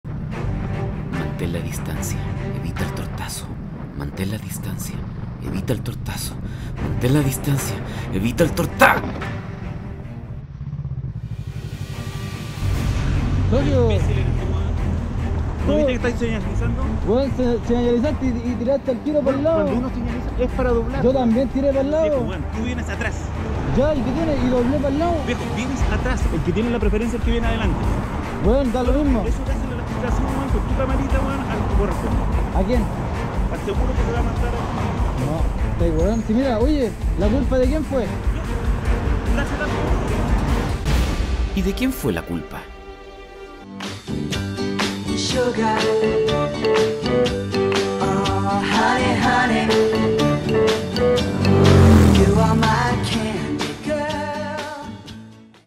MOTORISTA GOLPEA A AUTO. Mantén la Distancia, Evita el Tortazo.
Hace tiempo un seguidor nos envió este registro, lo que podemos decir al respecto es que hay dos Principios Legales que debemos siempre respetar:

1. Mantenerse atentos a las condiciones del tránsito del momento.(Art. 108)

2. Mantener una distancia razonable y prudente con el vehículo que nos antecede, que nos permita reaccionar frente a cualquier emergencia. (Art. 126).

¿Quién crees tu que fue el responsable en este caso?

¿SUFRISTE UN ACCIDENTE DE TRÁNSITO?